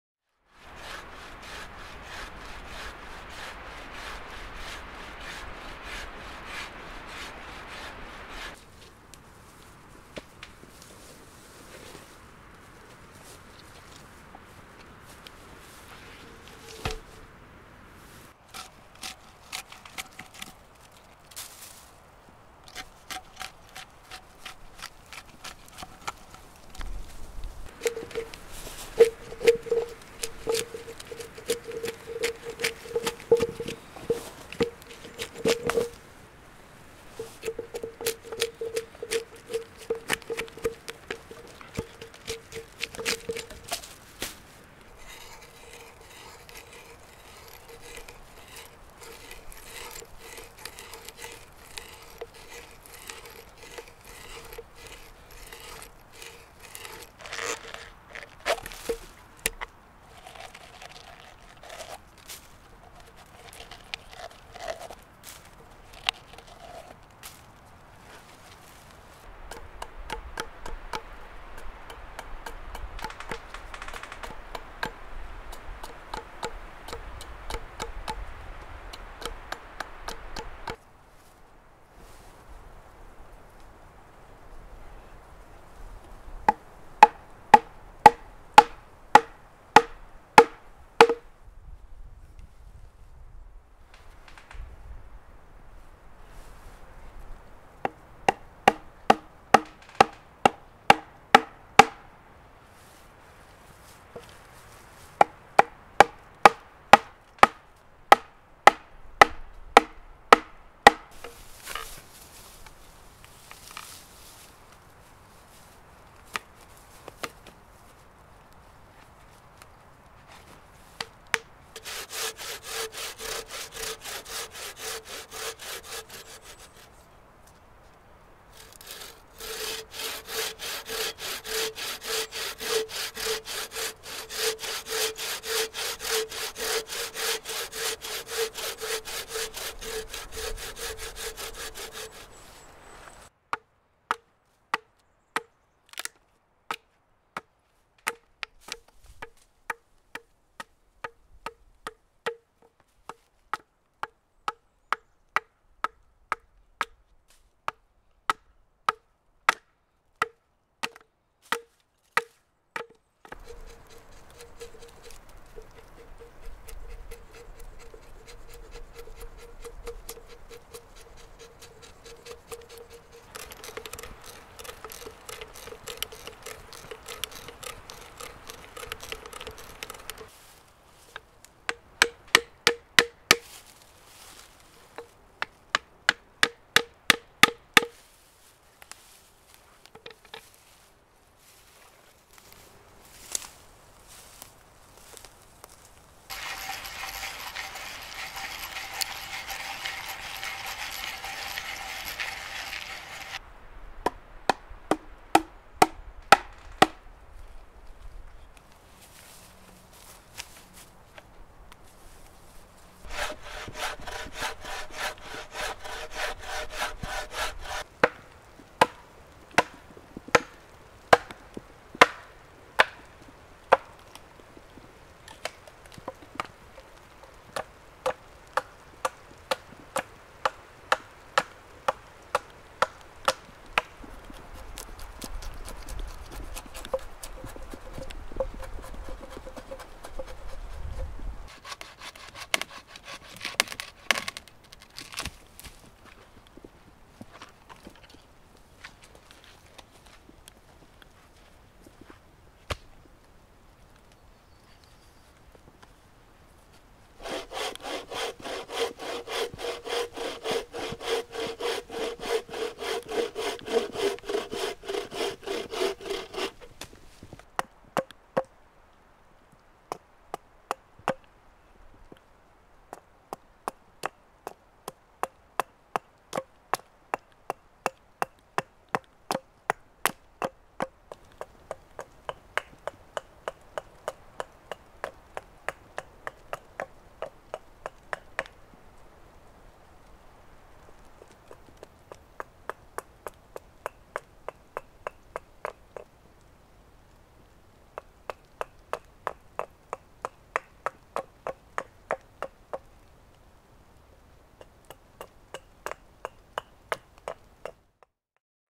Bushcraft Carving Camp
Just a short video showing the start of my permanent carving camp. This camp will allow me to complete more elaborate carving and woodworking projects.
So far the camp has a sawhorse which doubles as a table/flat working surface. I also made a carving bench for the hatchet work on my projects. Lastly I cut a stump which will serve as a chopping block and a seat while doing knife work.
I'm excited to finally have a proper carving camp and have many projects I will be starting soon.